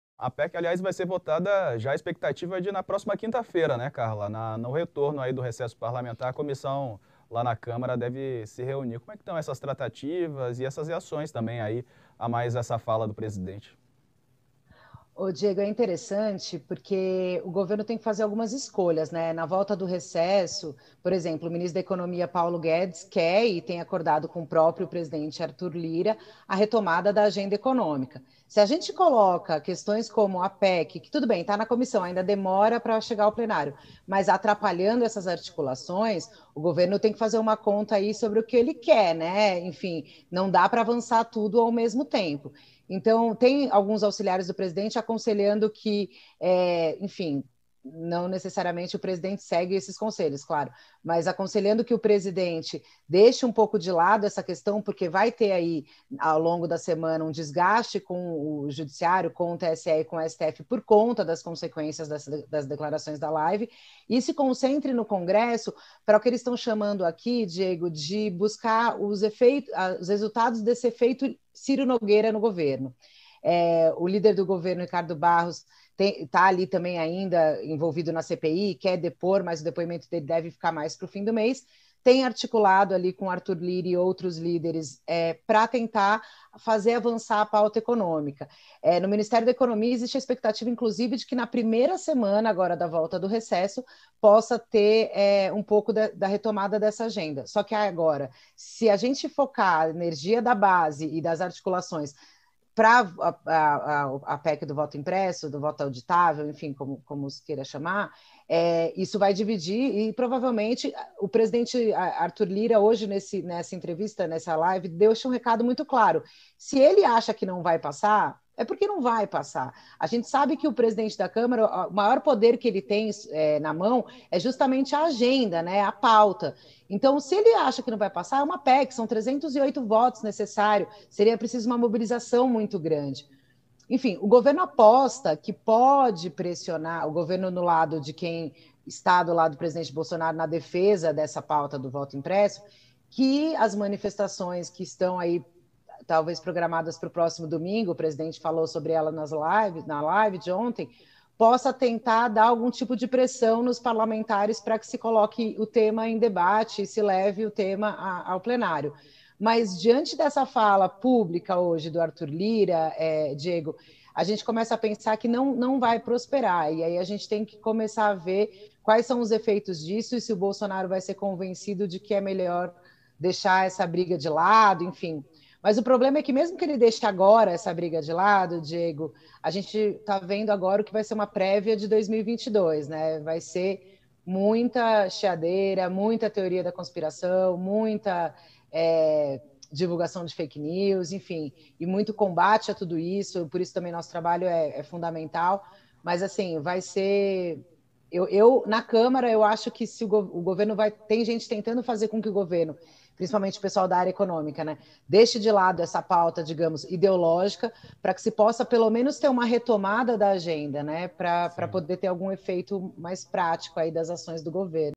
Governo Bolsonaro aposta em manifestações para pressionar PEC do voto impresso | Carla Araújo
Em participação no UOL News, a colunista Carla Araújo fala da articulação do governo federal no Congresso e diz que a gestão de Jair Bolsonaro (sem partido) está apostando na pressão das ruas para fazer avançar o voto impresso. Na live de ontem, o presidente citou ato marcado para o próximo domingo (1º) em defesa do que ele chama de voto "auditável e democrático". Para ela, o presidente dá prévia do que fará em 2022